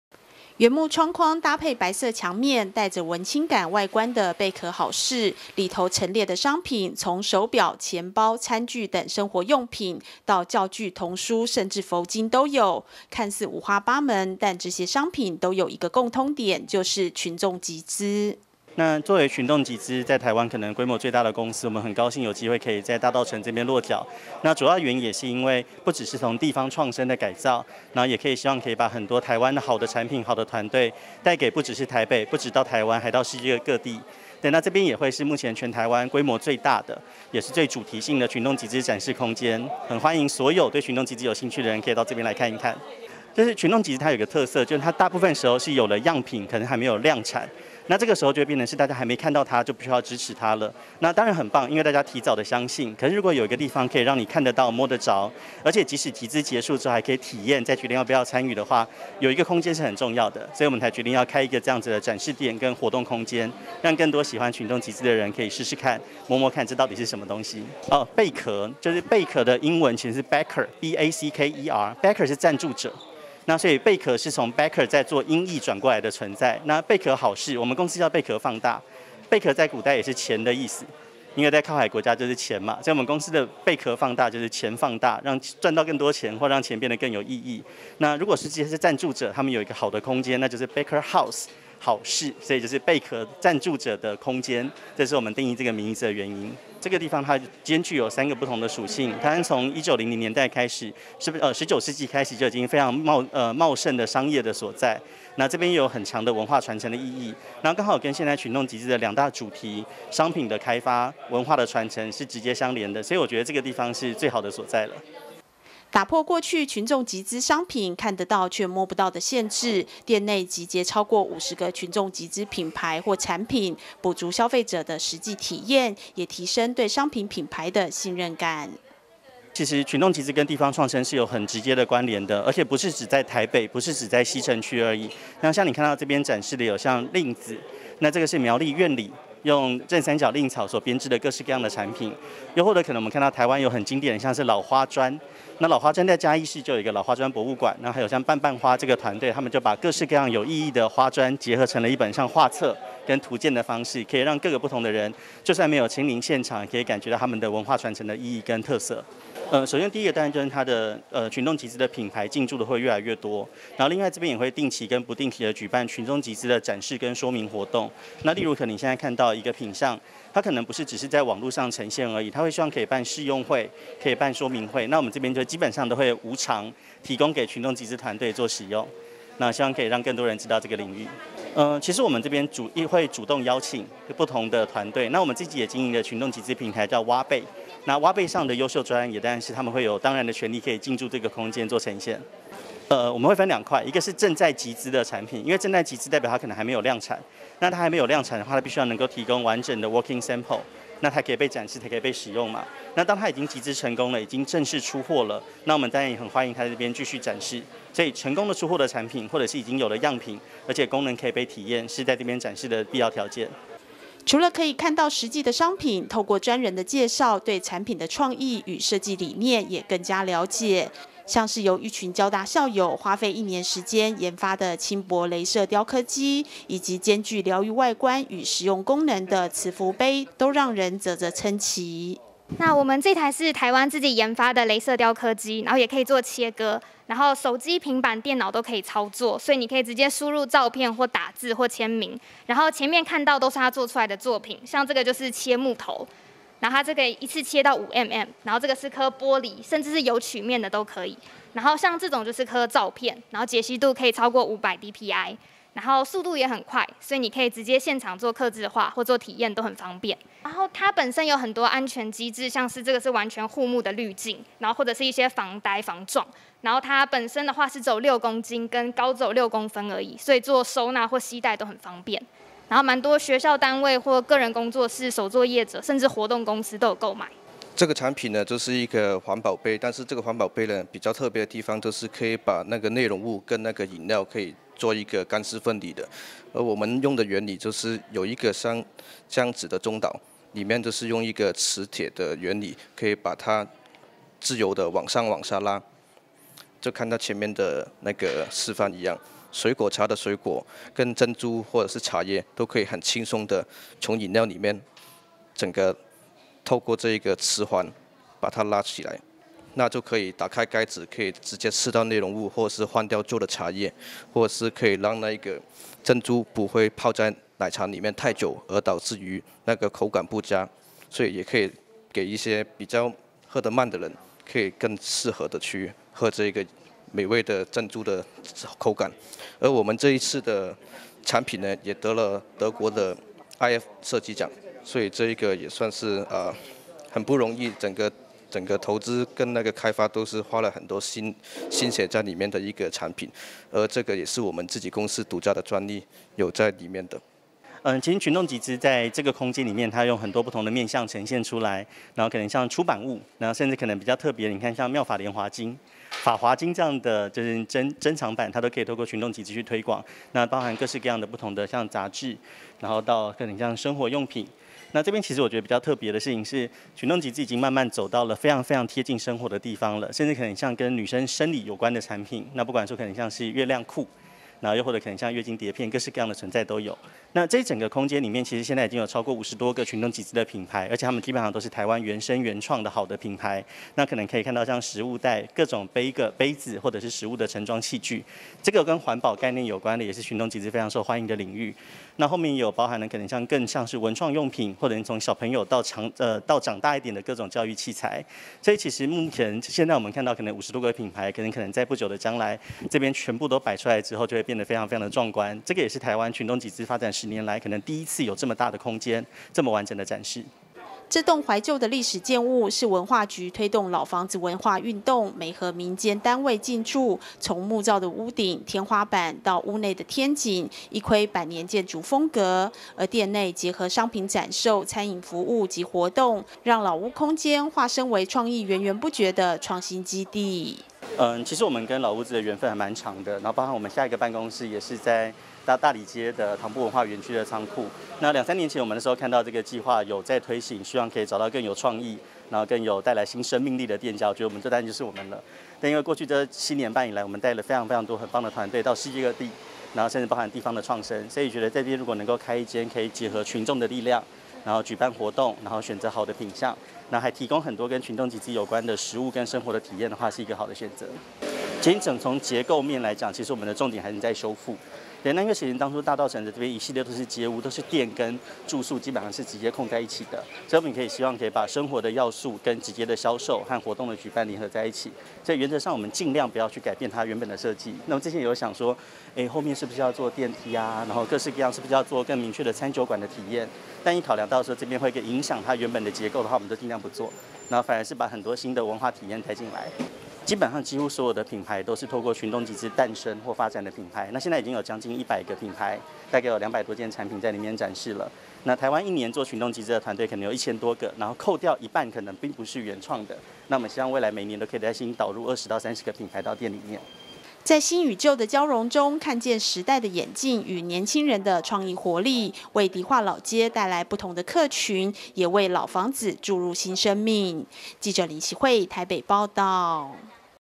群眾集資|「貝殼好室」 群眾集資商品創意基地
近年來群眾募資盛行，但大多是透過網路展示原生設計與創意作品，為補足與消費者的實體互動，全台最大的群眾集資顧問公司，在迪化街上成立實體店鋪「貝殼好室」，集結超過50個透過群眾集資誕生的品牌或產品，結合餐飲與沉浸體驗，打造老街中的創新基地。